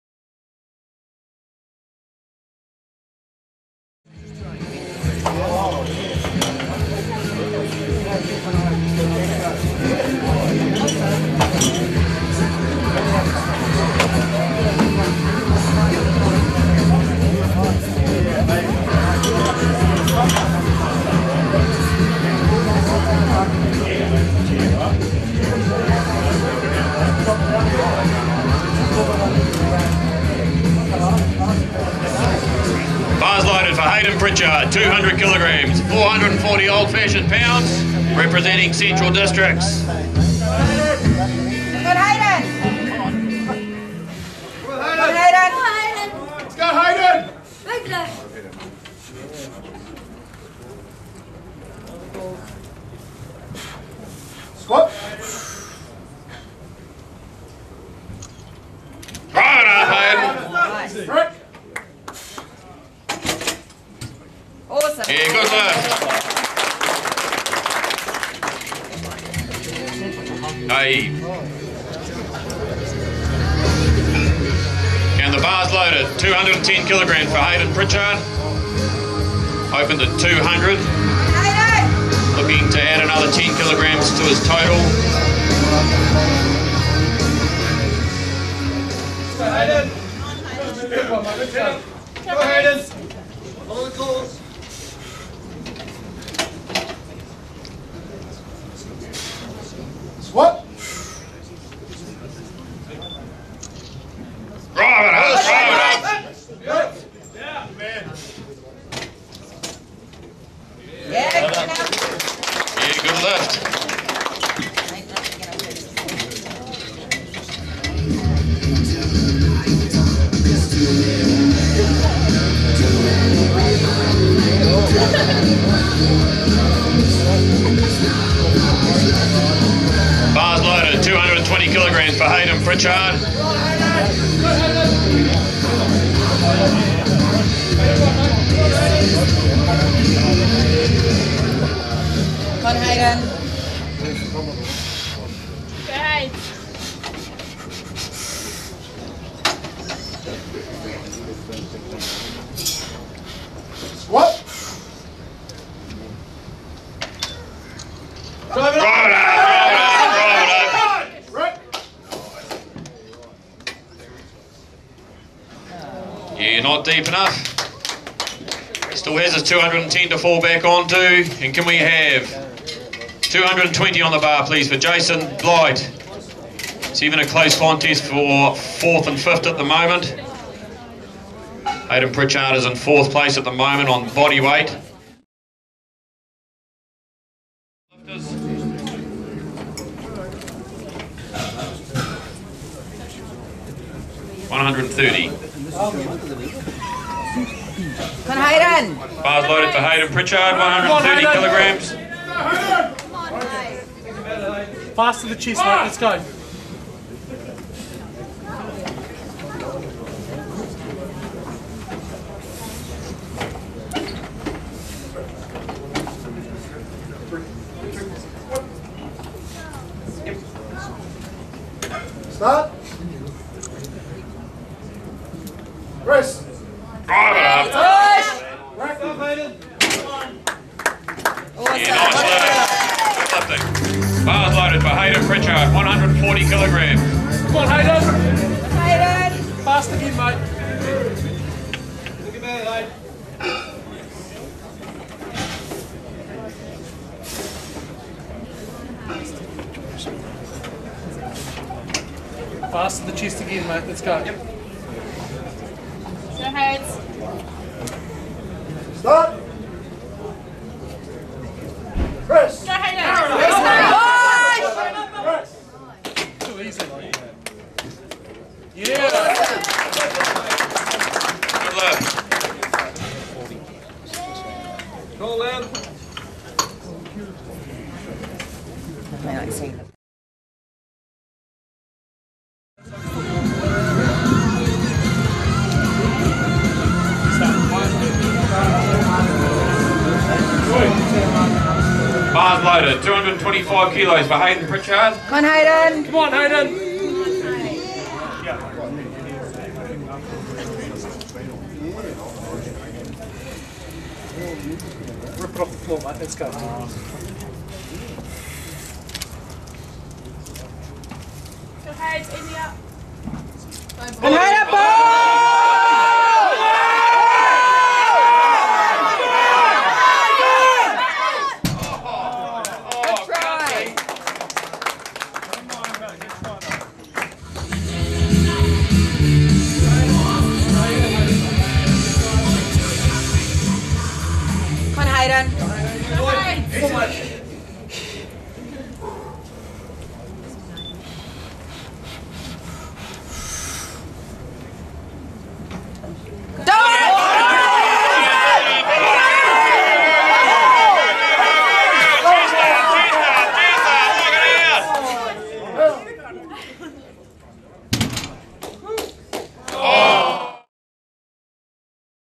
Hayden: National Powerlifting Champs
Hayden Pritchard, 83kg Class
Saturday, 6 August, 2011
Tauranga, NZ